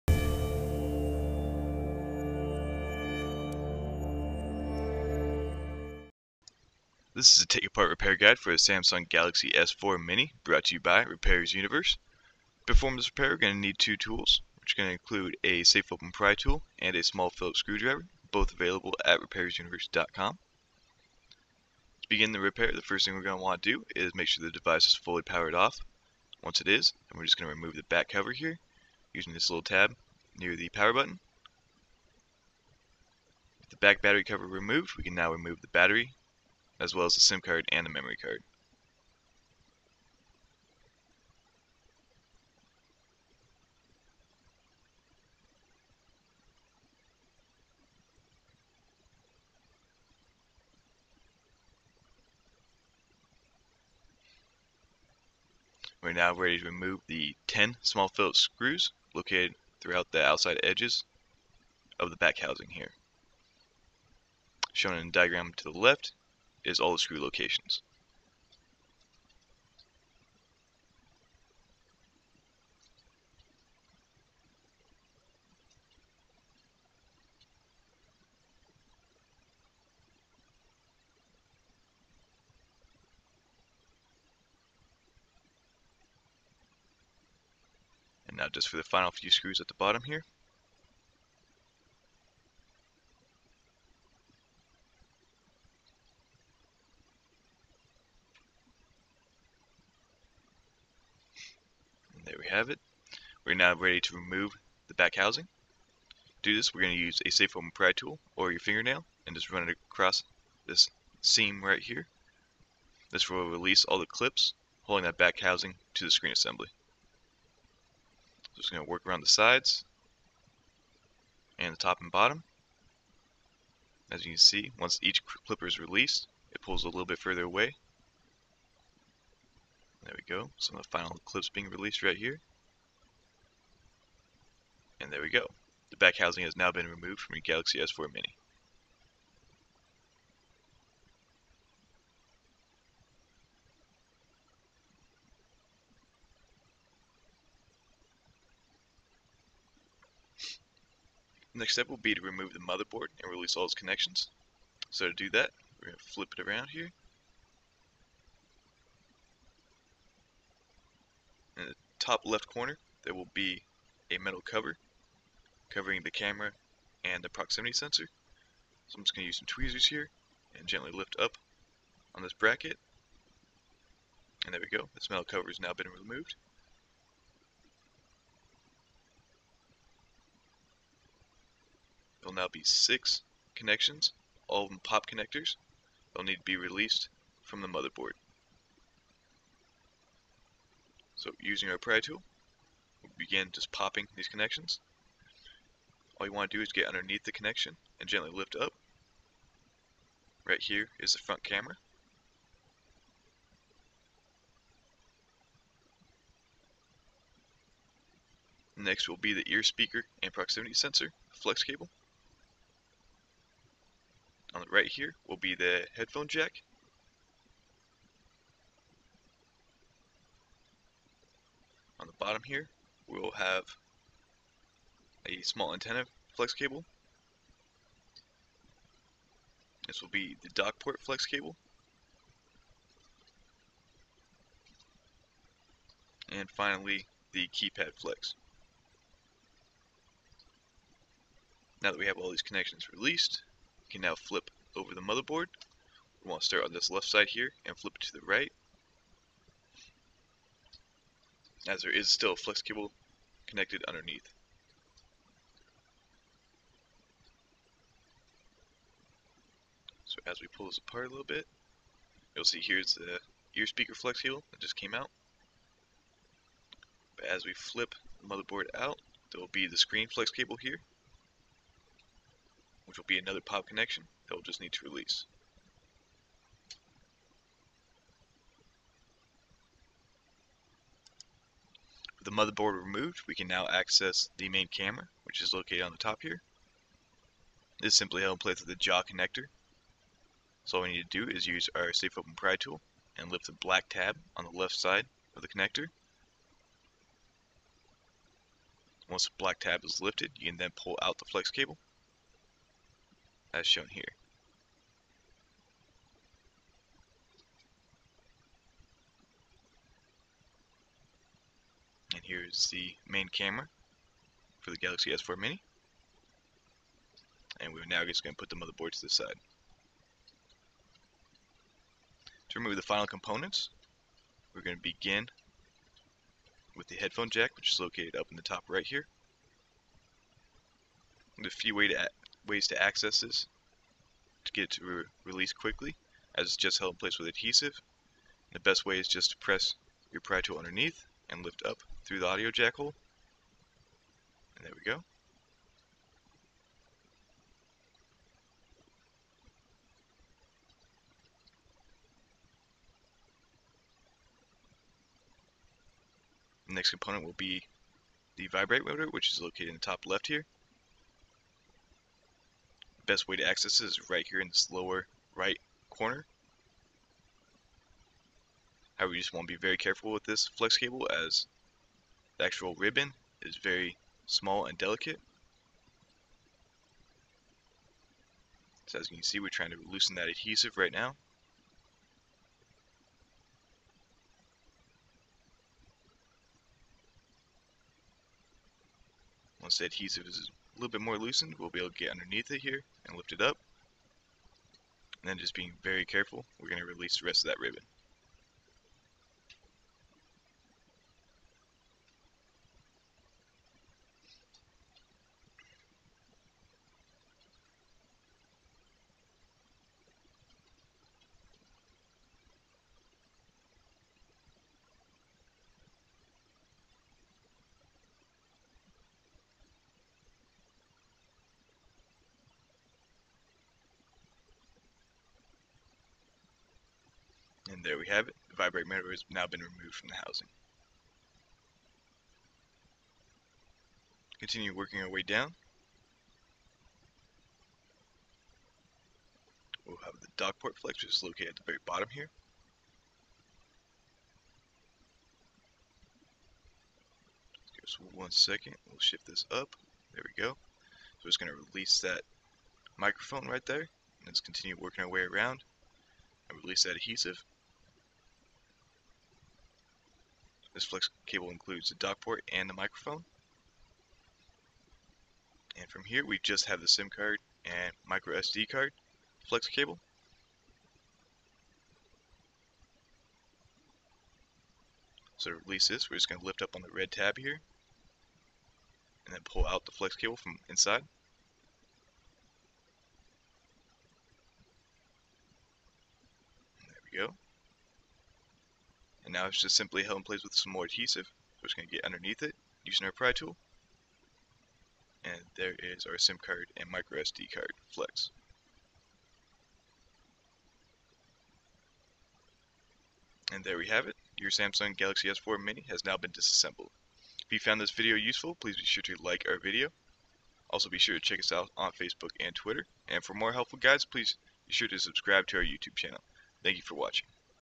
Samsung Galaxy S4 Mini Take Apart Repair Guide
- This teardown for the Samsung Galaxy S4 Mini will walk you through all the steps required to disassemble your device. If you would like to replace internal components or your screen on your S4 Mini, we will show you how in a detailed, step by step tutorial.